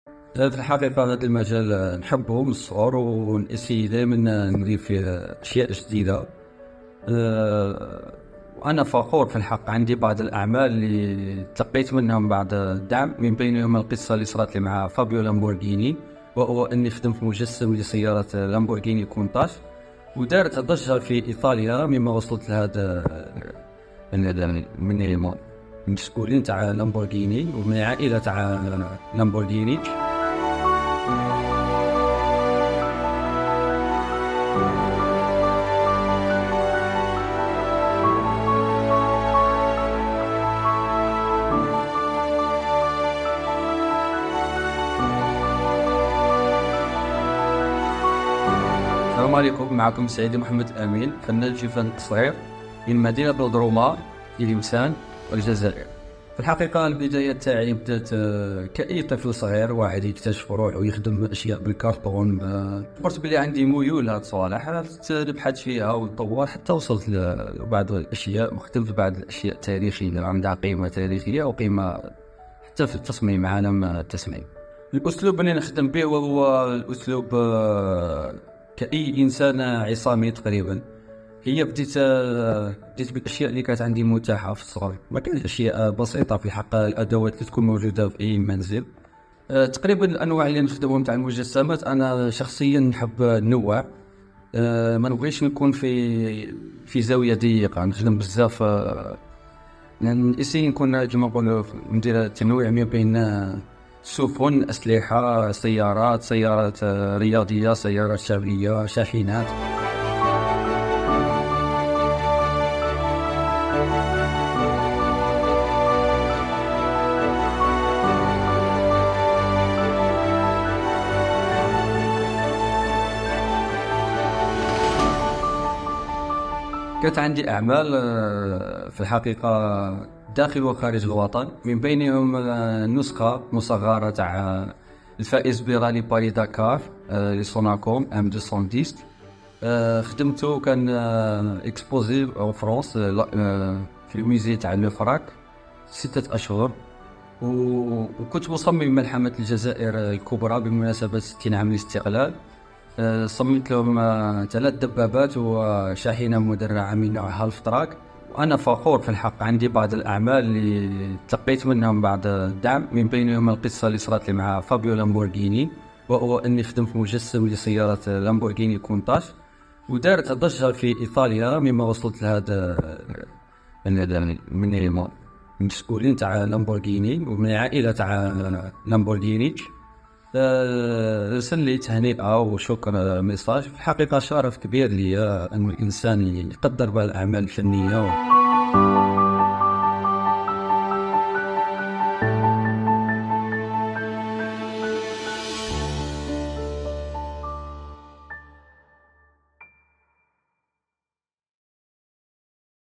محمد وفن التصغير.. إبداع من ورشة منزلية بالجزائر
سعيدي محمد ، شاب من مدينة تلمسان، يمتهن فن التصغير حيث يقوم بتشييد مجسمات دقيقة للسيارات والبواخر باستخدام أدوات بسيطة في ورشته المنزلية، يعرض هذه الأعمال في معارض متنوعة، مما جعل هوايته مصدراً للإبداع.. تابعوا معنا قصة محمد مع هذا الفن في هذا الفيديو:
ــــــــــــــــــــــــــــــــــــــــــــــــــــــــ

ــــــــــــــــــــــــــــــــــــــــــــــــــــــــ
لمعرفة أحدث المستجدات لحظة بلحظة.. يمكنكم متابعة البث المباشر لقناة الحدث 👇

ــــــــــــــــــــــــــــــــــــــــــــــــــــــــ